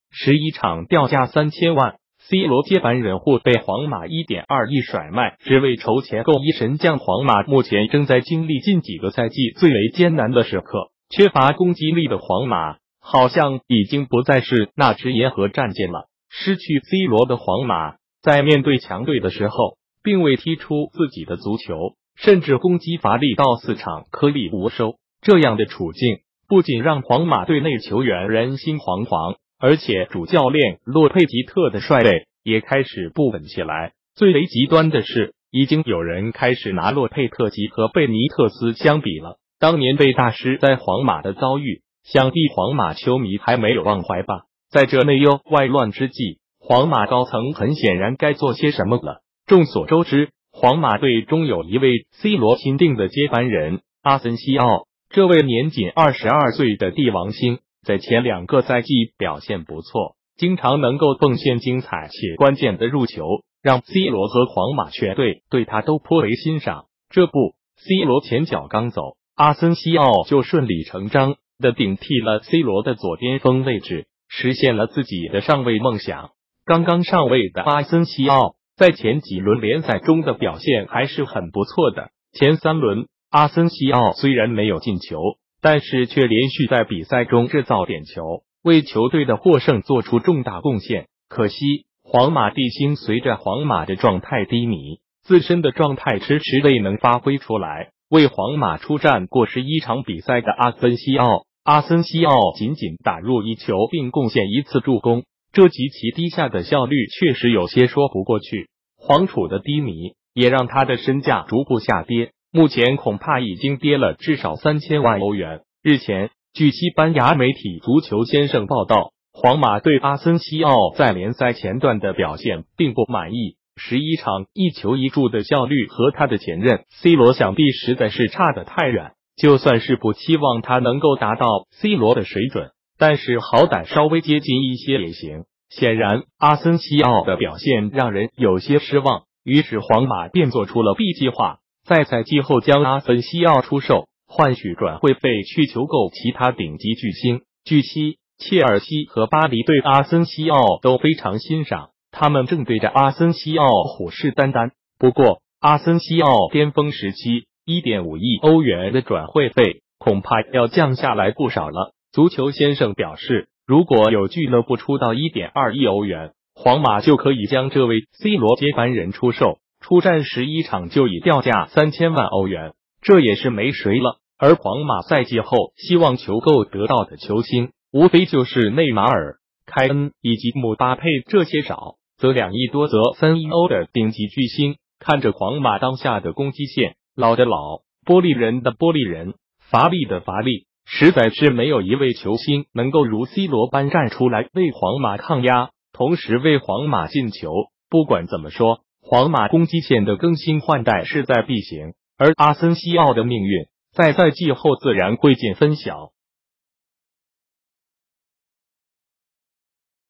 11場掉價3000萬？C羅接班人或被皇馬1.2億甩賣 只為籌錢購1神將
皇馬目前正在經歷近幾個賽季最為艱難的時刻，缺乏攻擊力的皇馬，好像已經不再是那支銀河戰艦了。失去C羅的皇馬，在面對強隊的時候，并未踢出自己的足球，甚至攻擊乏力到4場顆粒無收。